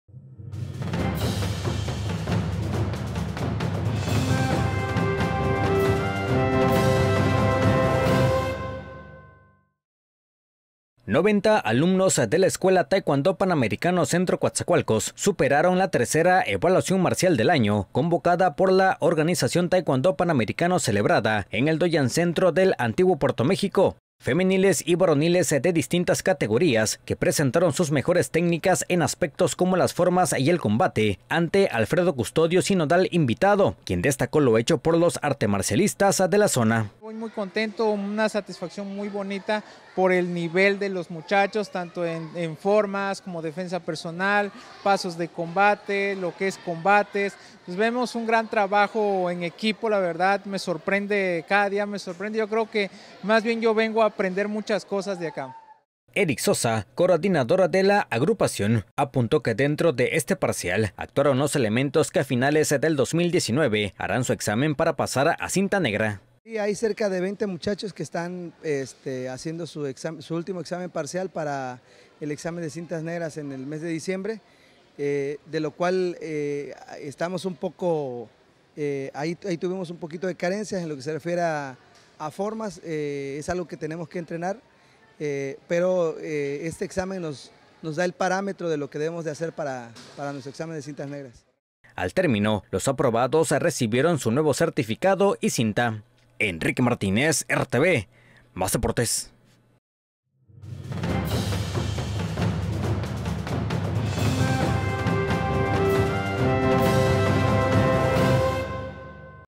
Artemarcialistas de Taekwondo Panamericano Centro encararon tercer examen marcial
Artemarcialistas de Taekwondo Panamericano Centro Coatzacoalcos, encararon tercer examen marcial del año.